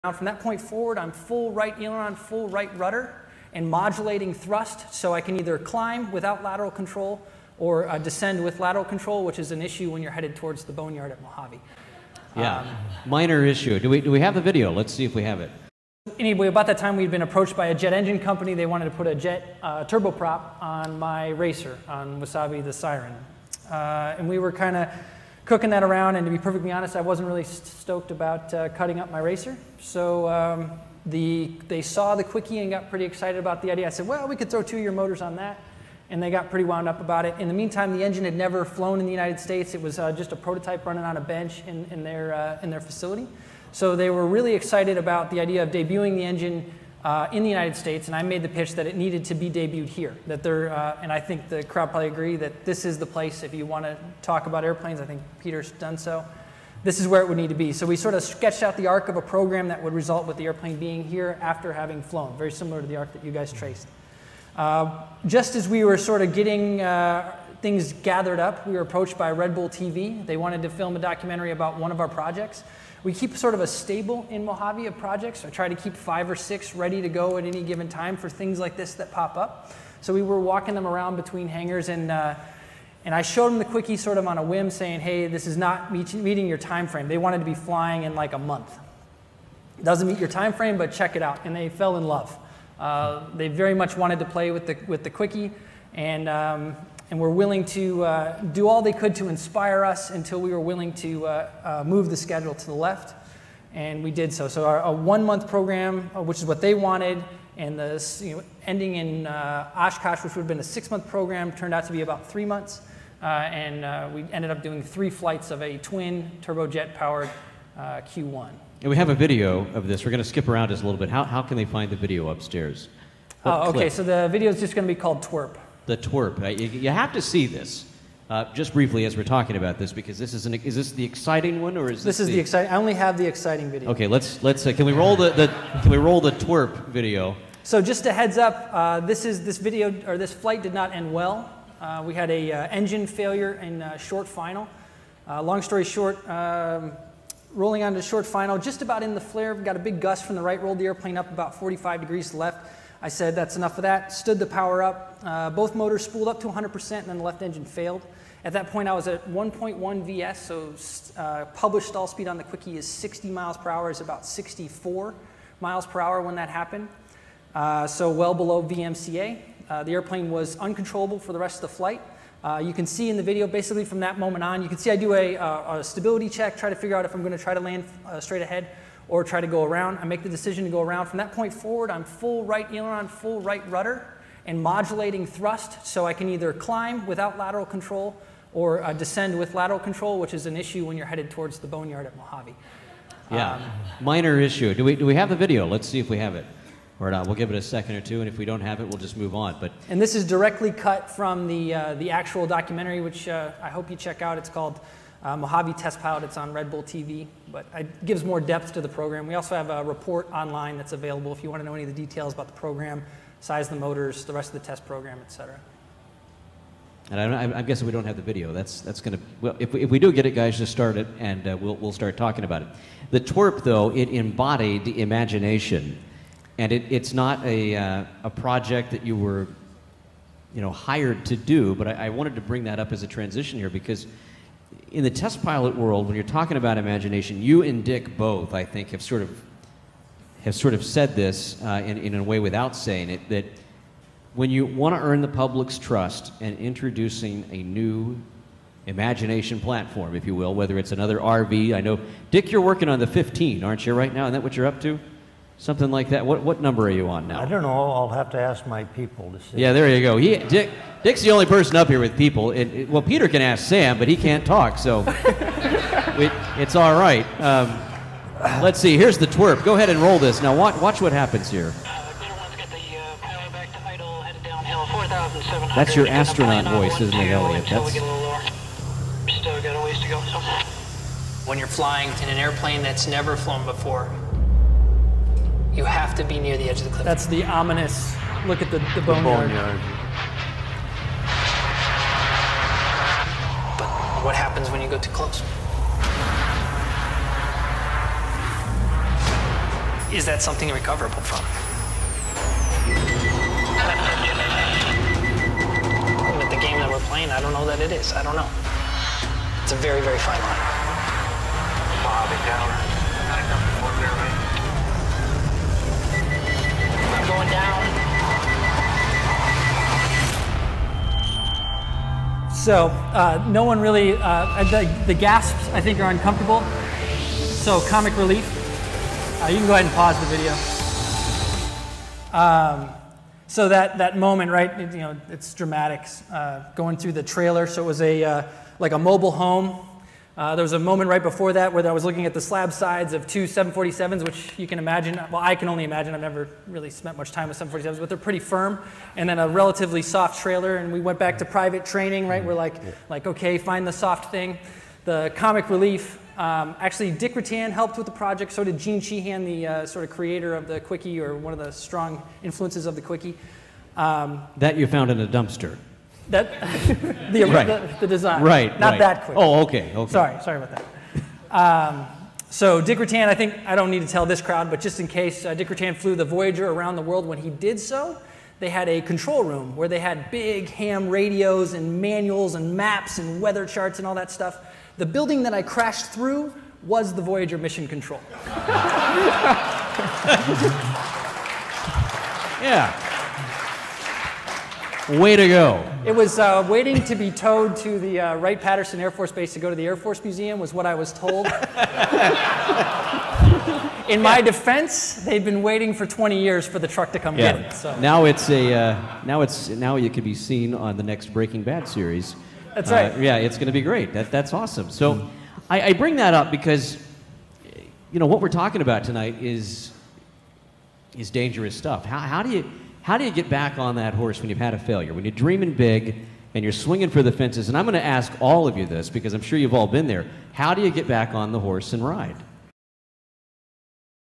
The TWERP Multi Jet Engine Canard Taildragger test bed crash at Oshkosh 2018
Twerp incident summary excerpt from the imagination in aviation panel at @EAA Airventure 2018. The panel featured Dick Vangrunsven of Vans Aircraft, Peter Sripol of @PeterSripol and was hosted by Matt Jolley. This was one of the few times I have spoken about the Twerp accident publicly and I think it went well.

airplanes are cool